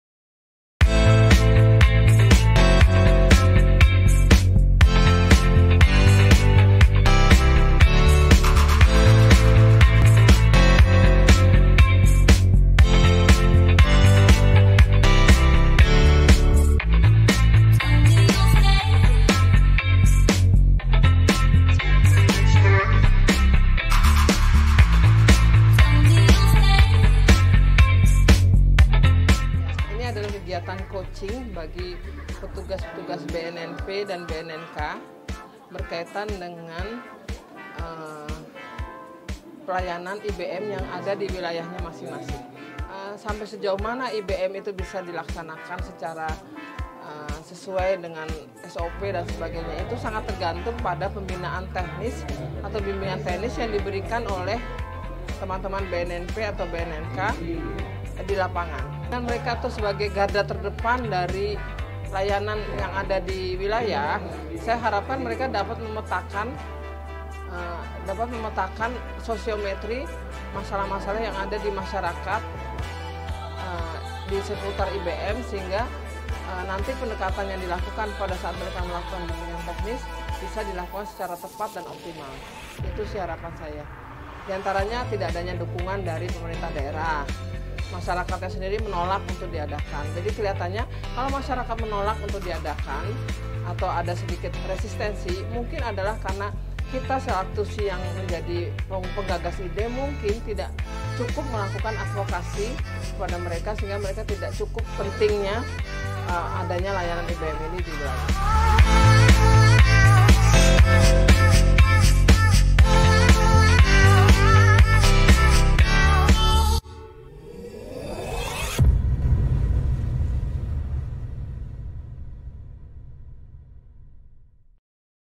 Tingkatkan Kapasitas Program IBM, BNN RI Lakukan Coaching kepada BNNP dan BNNK
Permasalahan minimnya aksesibilitas tempat rehabilitasi di Indonesia merupakan salah satu tantangan yang dihadapi BNN RI. Sebagai salah satu upaya untuk mengatasi tantangan tersebut,BNN RI melalui Deputi Bidang Rehabilitasi  terus meningkatkan program Intervensi Berbasis Masyarakat (IBM) baik dalam aspek kuantitas maupun kualitas di seluruh penjuru nusantara. Pada tahun 2022, telah dibentuk 307 unit IBM di seluruh Indonesia.

Dalam rangka memastikan program IBM berjalan sesuai dengan pedoman, Direktorat Penguatan Lembaga Rehabilitasi Komponen Masyarakat (PLRKM) Deputi Bidang Rehabilitasi BNN RI melaksanakan kegiatan Coaching Petugas Rehabilitasi BNNP dan BNN Kabupaten/Kota Dalam Meningkatkan Kapasitas Program IBM, di Bekasi secara luring dan daring. Adapun peserta kegiatan pada tahap 1,  yang digelar mulai 27 hingga 30 September 2022  ini berasal dari 77 Satker di 14 Provinsi.

Kegiatan Coaching Petugas Rehabilitasi BNNP dan BNN Kabupaten/Kota Dalam Meningkatkan Kapasitas Program IBM tahap 1 ini dibuka oleh Deputi Rehabilitasi BNN RI, Dra. Riza Sarasvita, M.Si, MHS, Ph.D. pada Selasa (27/9). Ditemui usai membuka kegiatan, Deputi Rehabilitasi mengungkapkan bahwa kegiatan ini sangat bernilai penting, karena sejauh mana program IBM ini dapat dilaksanakan sesuai dengan SOP, akan tergantung pada bagaimana pembinaan teknis dari petugas BNNP atau BNNK di lapangan.

“Karena mereka garda terdepan dalam memberikan layanan di wilayah, saya harapkan mereka dapat memetakan sosiometri masalah sosial yang ada di masyarakat di seputar IBM sehingga nanti pendekatan yang akan dilakukan bisa dilakukan secara tepat dan optimal,” imbuh Deputi Rehabilitasi.

Kepada seluruh petugas BNNP dan BNNK yang mengikuti kegiatan secara daring, Deputi Rehabilitasi memberikan semangat dan motivasi untuk dapat menyerap seluruh materi dengan sungguh-sungguh serta mencurahkan pemikirannya melalui diskusi yang dinamis. Menurut Deputi, para petugas di daerah adalah pejuang rehabilitasi yang berhadapan  langsung dengan masyarakat secara nyata di wilayah. Oleh karena itulah keterampilan dan pengetahuan sangat dibutuhkan oleh para petugas, sehingga  pelaksanaan program IBM di wilayah berjalan sesuai harapan.

Dalam kesempatan ini, Deputi Rehabilitasi juga menyampaikan bahwa selain untuk meningkatkan kemampuan petugas bagi petugas BNNP & BNNK/Kota seluruh Indonesia dalam menjalankan fungsi pembinaan terhadap unit IBM di wilayah masing-masing, kegiatan ini juga bertujuan untuk melakukan pemantauan tata kelola serta kegiatan dan layanan pada unit IBM binaan BNNP dan BNNK seluruh Indonesia.

Untuk meningkatkan kemampuan para petugas di wilayah, Direktorat PLRKM BNN RI memberikan berbagai materi mulai dari pengenalan tentang IBM, Followership program IBM, pembinaan unit IBM, hingga ke digitalisasi IBM.

Melalui serangkaian materi tersebut, para peserta diberikan bimbingan dan pendampingan secara teknis sehingga nantinya mereka terampil mengatasi kendala yang dihadapi dan memiliki keyakinan dalam memberikan layanan pada masyarakat.

Sementara itu, Direktur PLRKM BNN RI, dr. Amrita Devi, Sp.KJ.,M.Si menekankan agar target realisasi operasional IBM terus dikejar. Terhadap segala kendala-kendala yang dihadapi di tahun ini, Direktur PLRKM meminta agar dilakukan evaluasi yang komprehensif sehingga program IBM dapat terlaksana sesuai dengan output dan outcome.